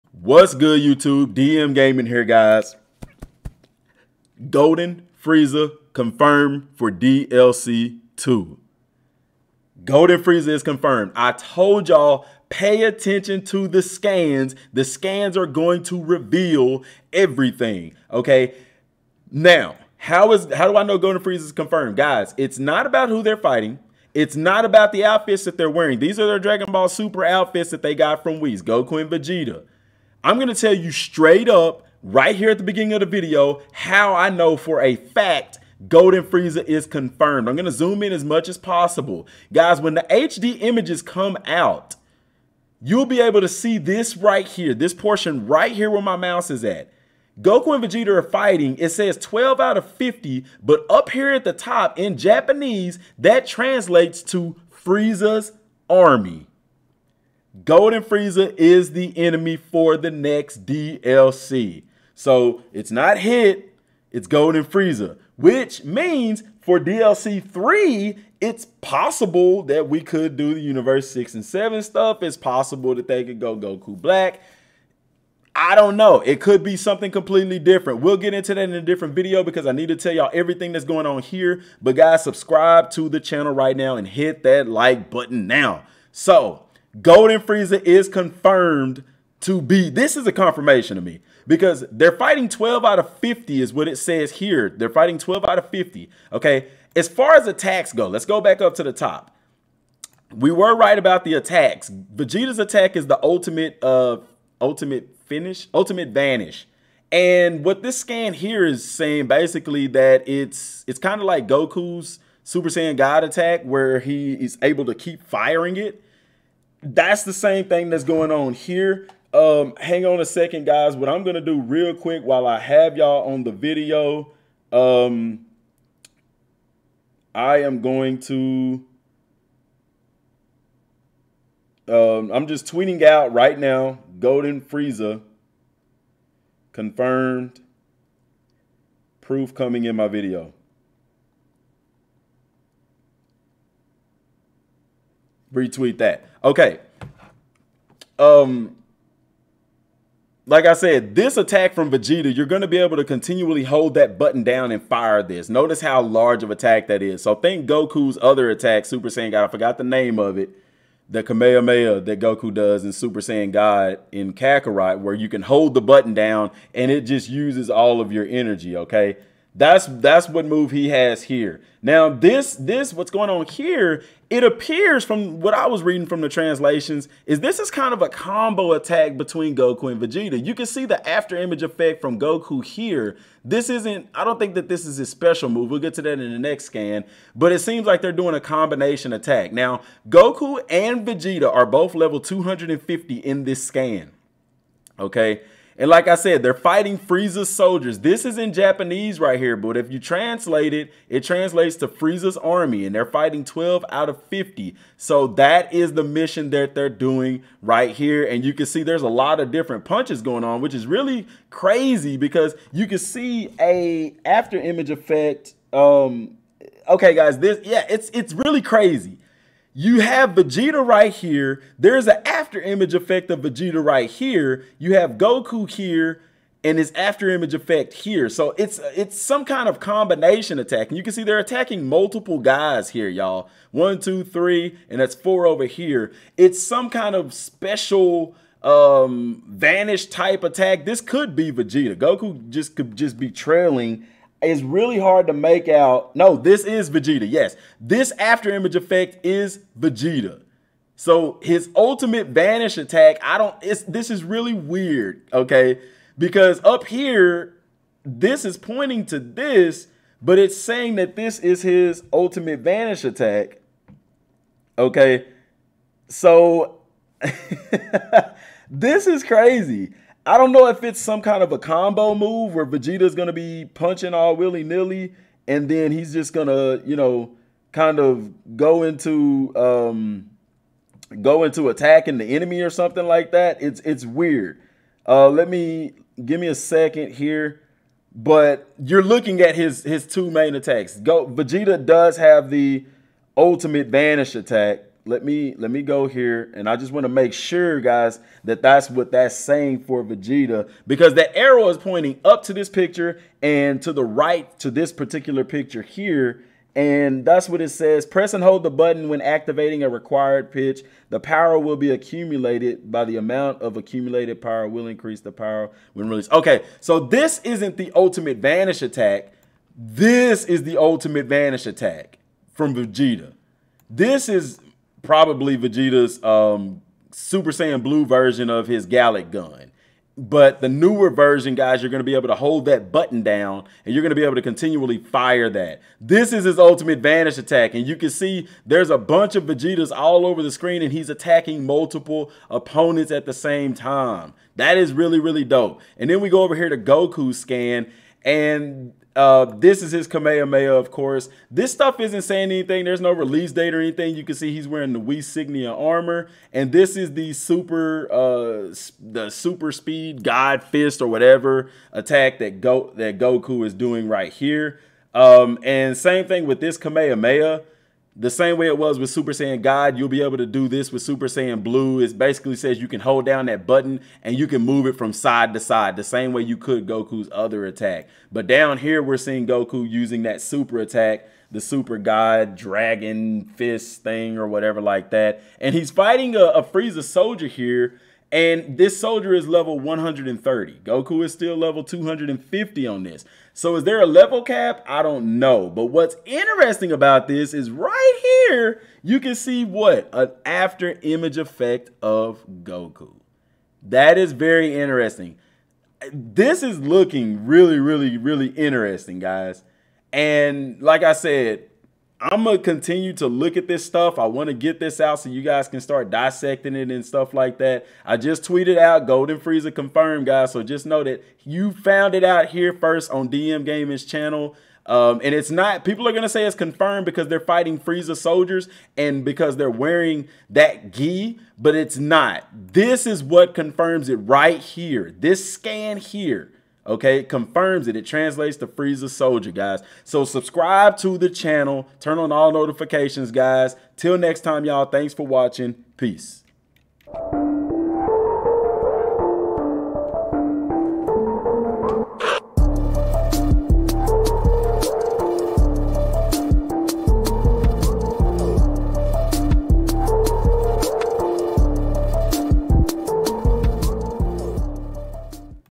GOLDEN FRIEZA CONFIRMED Dragon Ball Z Kakarot V-jump Scans
Based on the scans you will see how golden frieza is confirmed for dragon ball z kakarot.

Extra 5% Order Over $49, Code: Anime
Extra 7% Order Over $79, Code: Senpai
Extra 10% Order Over $99, Code: Daisuki